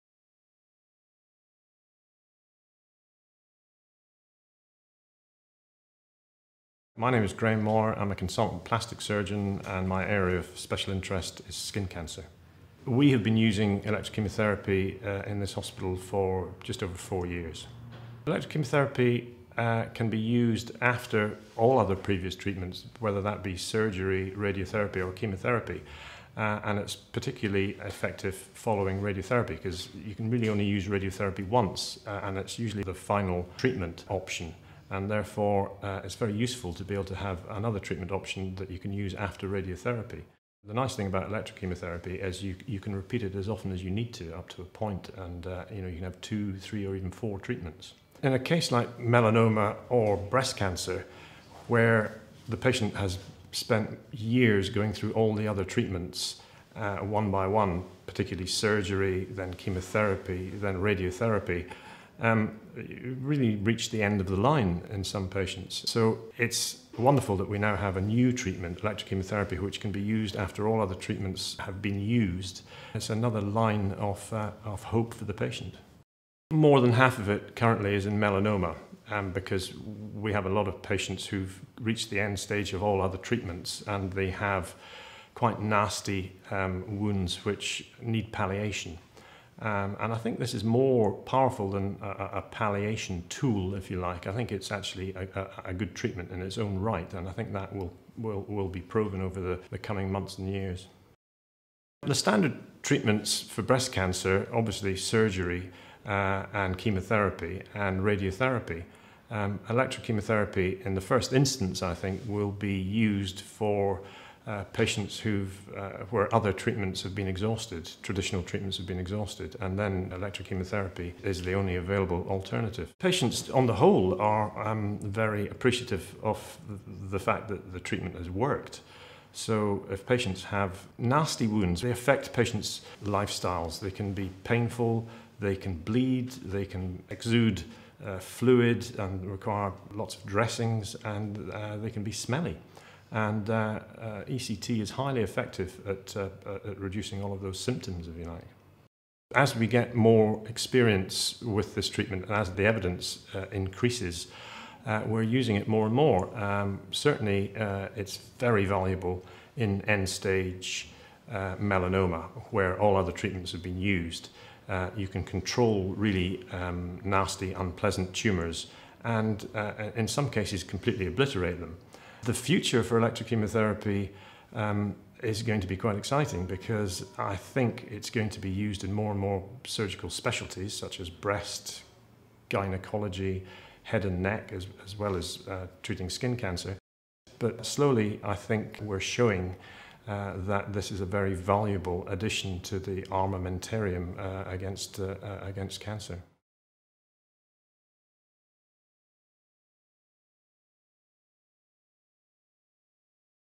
Surgeon's experience - Electrochemotherapy Patient Case Study
This video shows a surgeon's experience of using Electrochemotherapy in clinical practise.